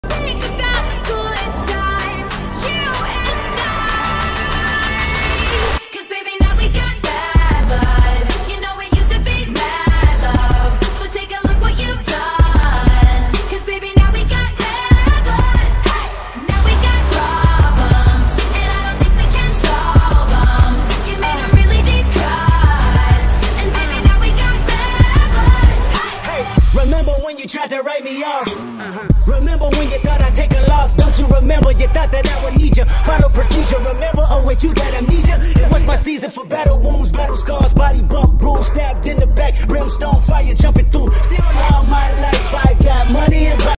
Lena Dunham Now Proud to Run, Says She Used to Do It Looking "Like a Wounded Baby Pterodactyl"
Go, river! Lena Dunham has overcome a life-long hate of running, and with the assistance of a private trainer has began to feel proud, instead of embarrassed, once she pounds the streets in her exercising gear.The Girls star took to Instagram on Wednesday, July 29, to share a paparazzi image of herself operating up a sweat in skin-tight blue lycra whereas taking a jog. And whereas she's no fan of uninvited photos, the player wished to share the pride she felt on seeing the snap, not least as a result of running accustomed be one thing she may hardly bear to try and do."Not typically one to post a paparazzi shot however this fills American state proudly," the 29-year-old player captioned the photograph. "Basically my whole life I even have unloved running and run sort of a wounded baby flying reptile. it absolutely was embarrassing and honestly I didn't trust myself to flee a burning building or perhaps move briskly towards a buffet," she continued .